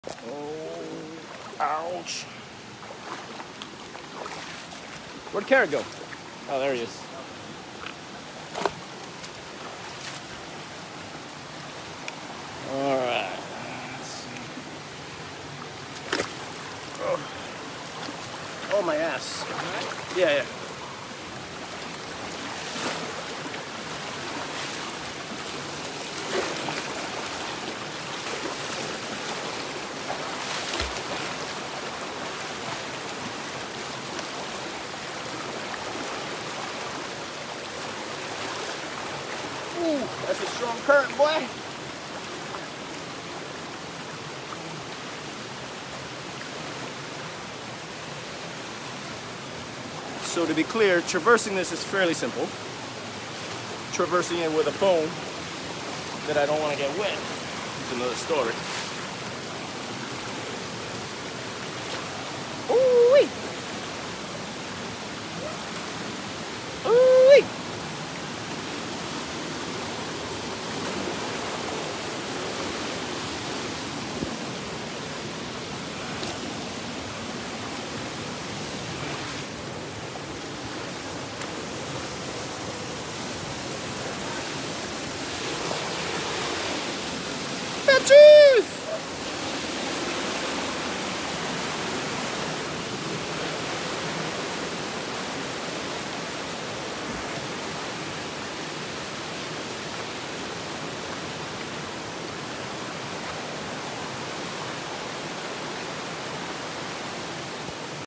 Looking for a waterfall
so its easier going through the water than on land